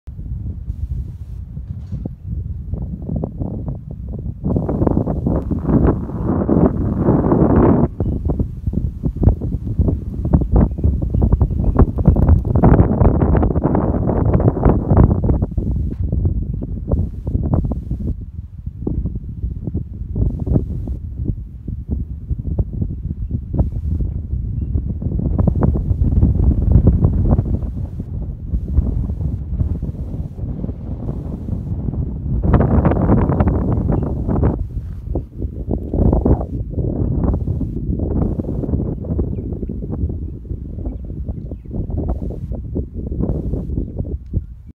Campsite, De Riet, Namibia
A cave with prehistoric art as camping site.